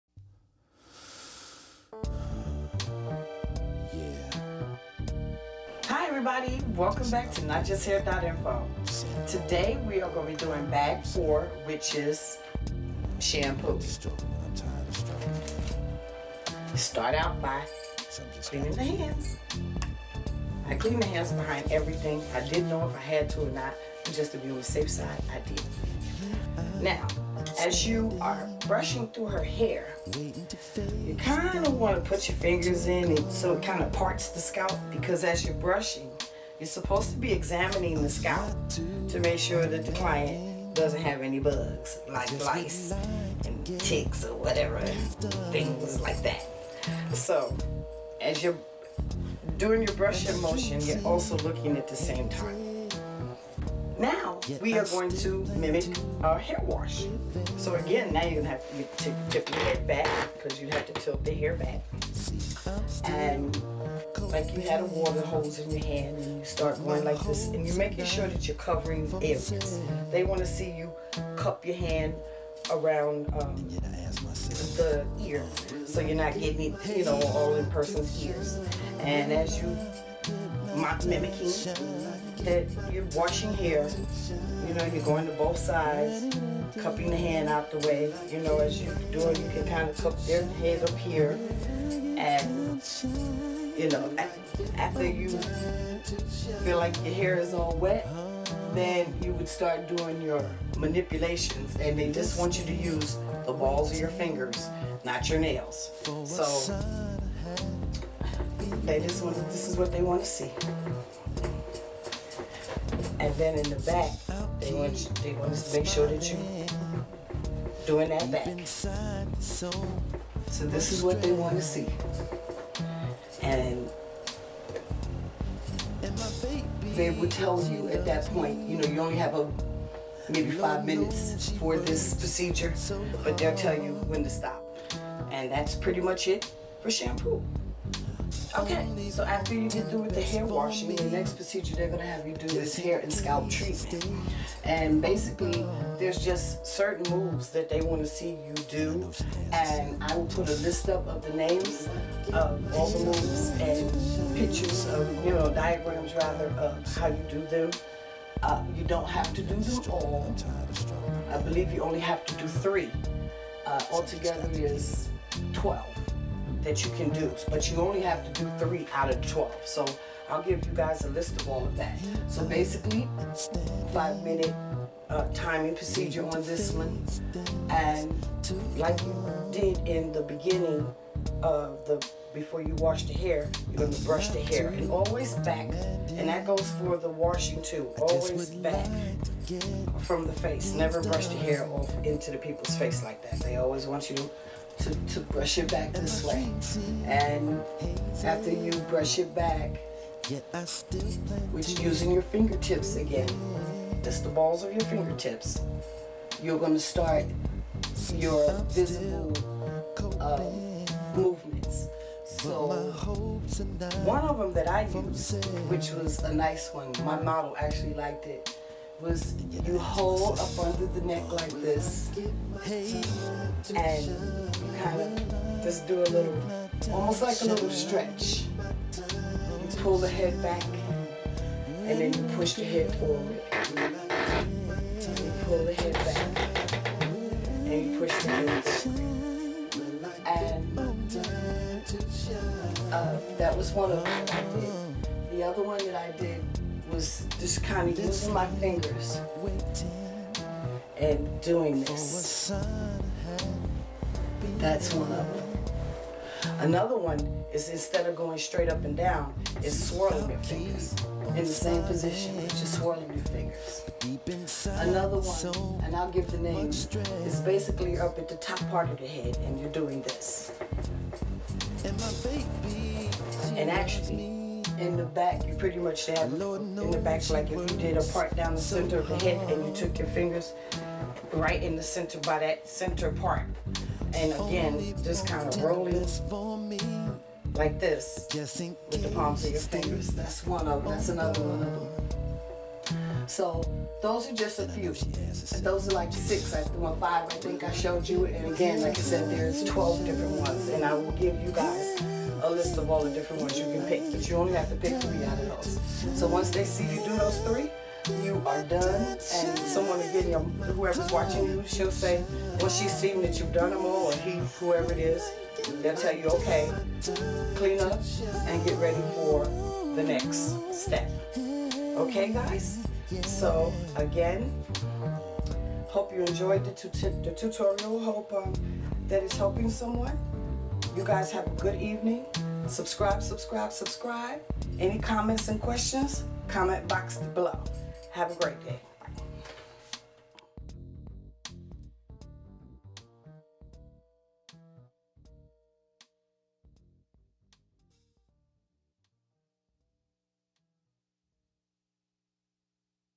NJ State Board Cosmetology Exam Help: Shampoo & Scalp Treatment Bag #4
This video is about Bag #4 Shampoo & Scalp treatment